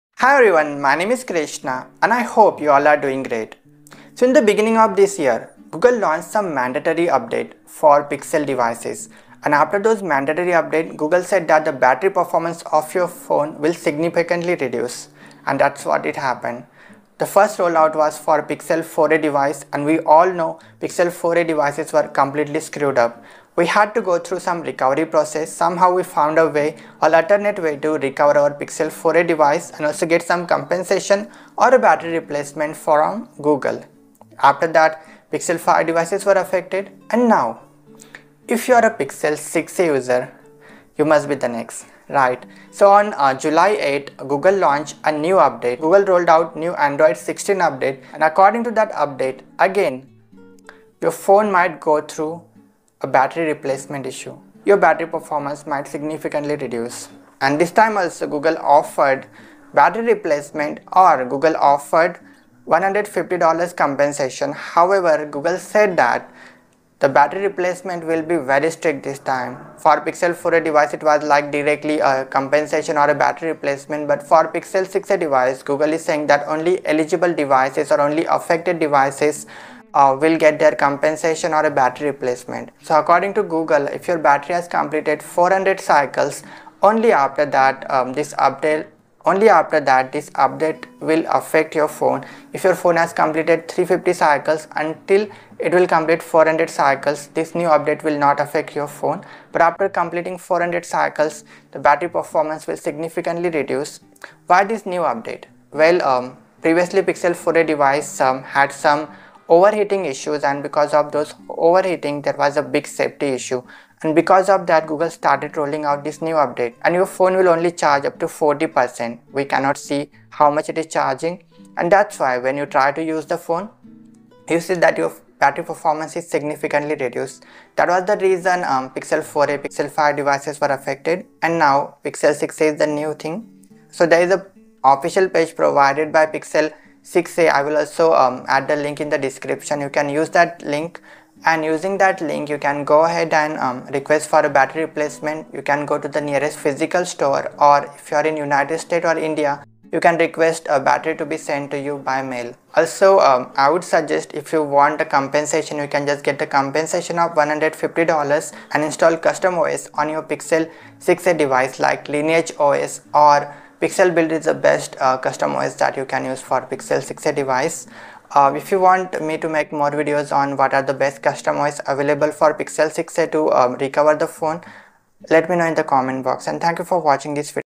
After Pixel 4a and Pixel 6a device battery issue | Install Lineage os to recover your pixel 6a
Recover Pixel 6a devices from Google new android 16 update

First Pixel 4a now Pixel 6a devices are going through battery issue after Google's new update

Pixel 9a